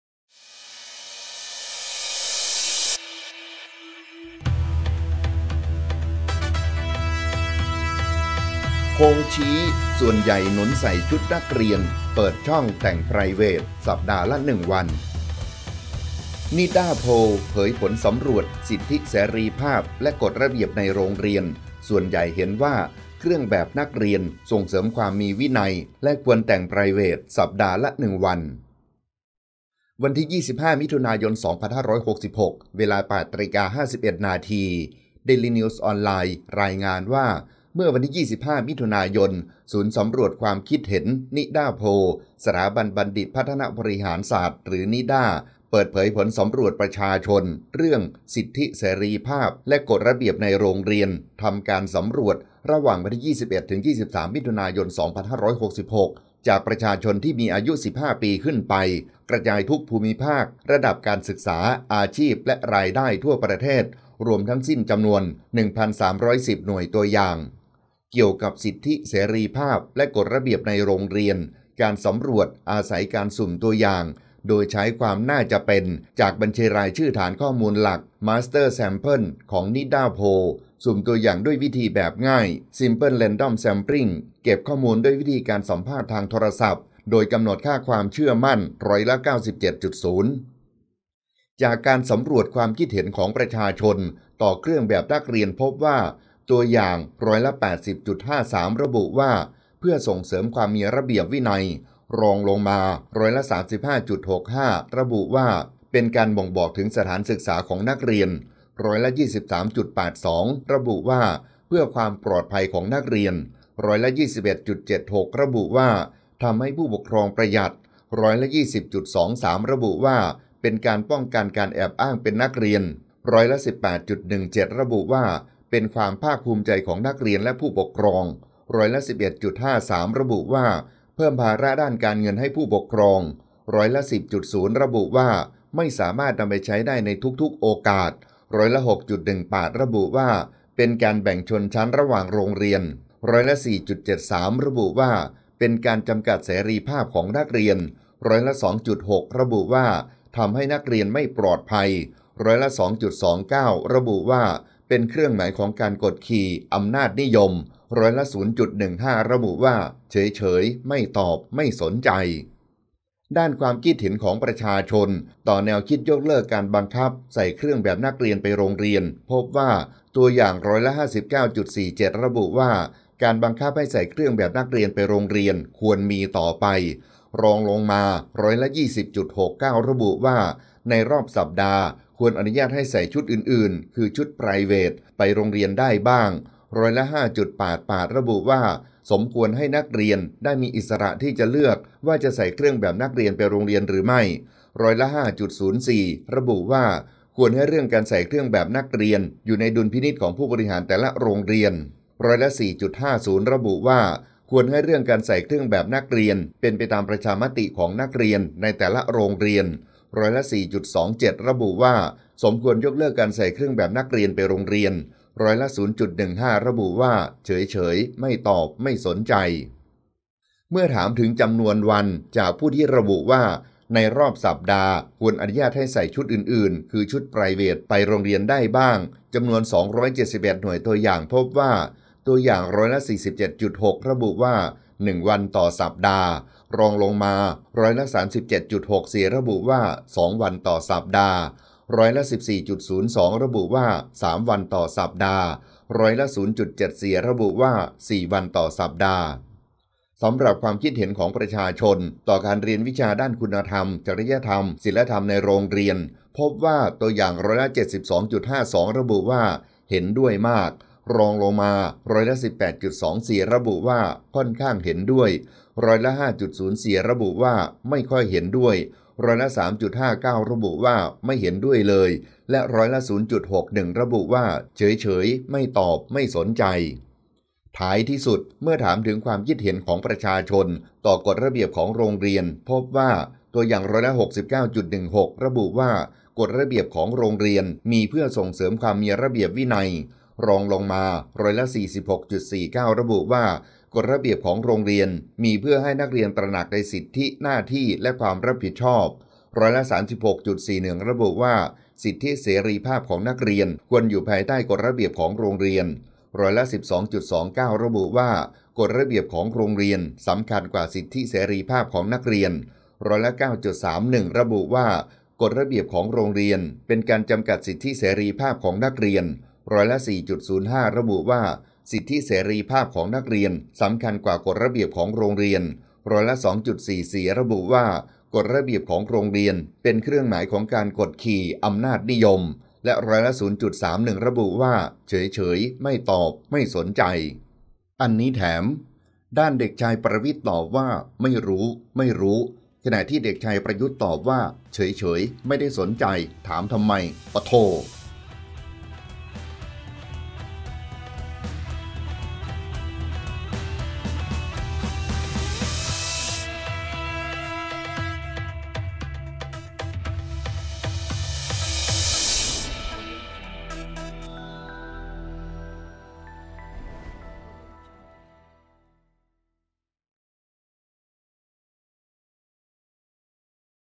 โพลชี้! ส่วนใหญ่หนุนใส่ 'ชุดนักเรียน' เปิดช่องแต่ง 'ไปรเวท' สัปดาห์ละ 1 วัน
ขอบคุณที่มา : dailynews , นิด้าโพล